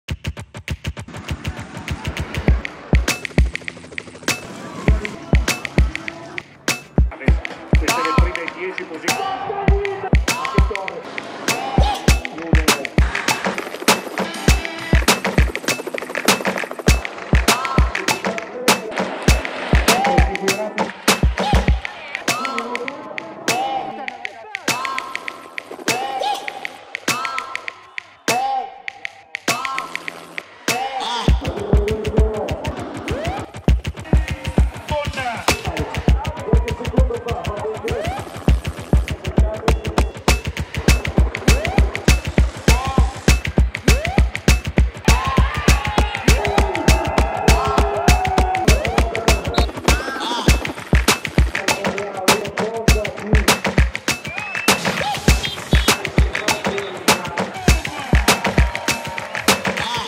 Entire 2018 Giro in 60seconds!
Giro d'Italia withdrawals? We've got the entire race, right here, in 60seconds. Presented by Easton Cycling